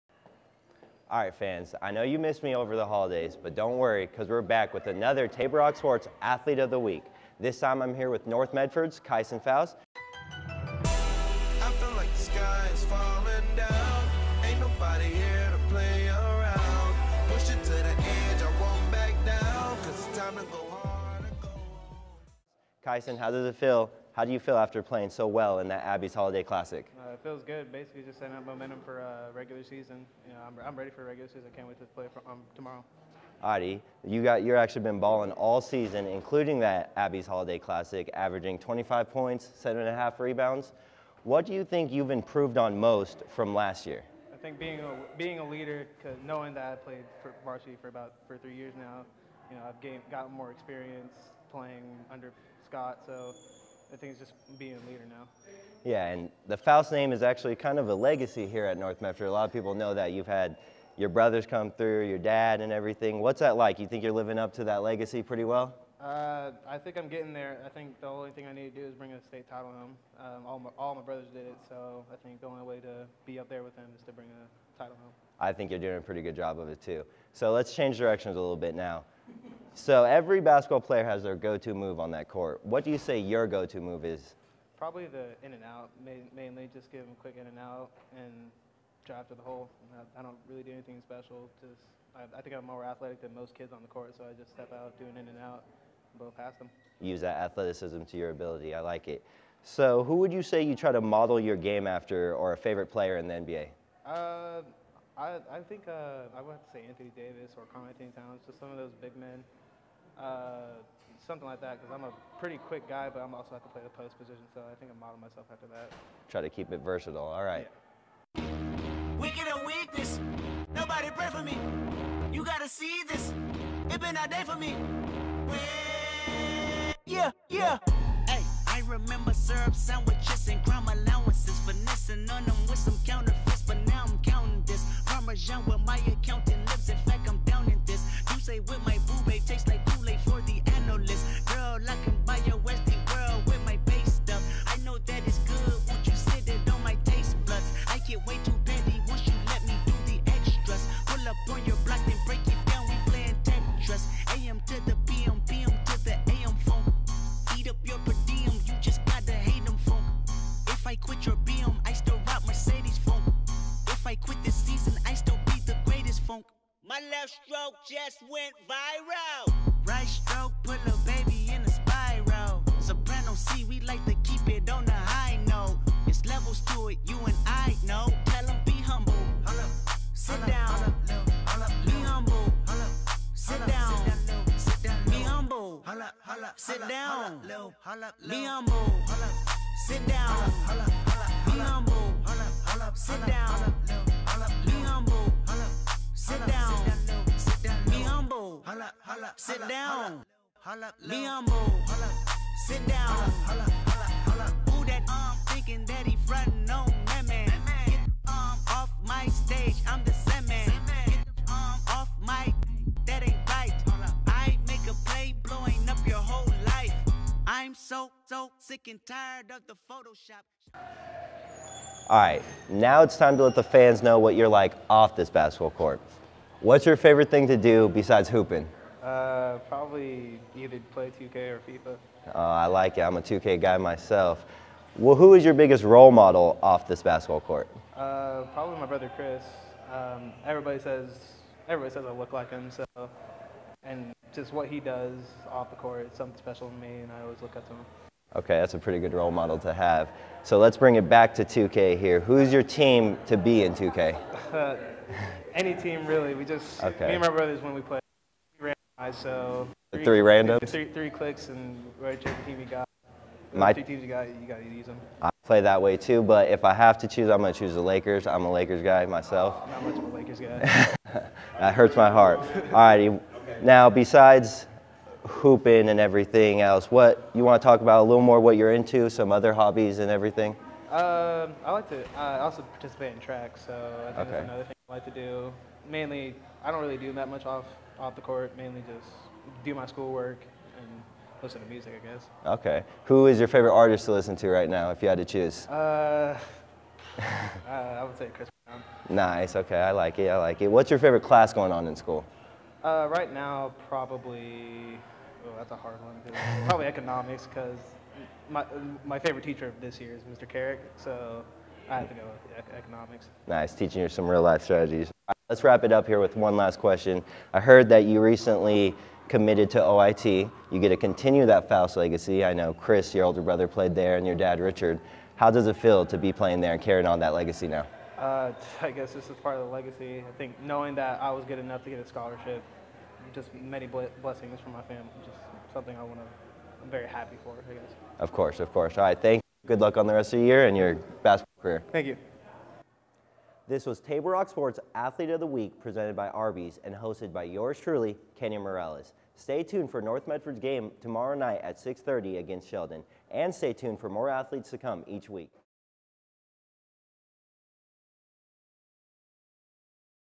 Athlete of the Week | Kaison Faust | North Medford Boys Basketball
Kaison Faust helped lead North Medford to the State Tournament last year and he is back this year as a senior. He is averaging 25 points per game and 7.7 rebounds.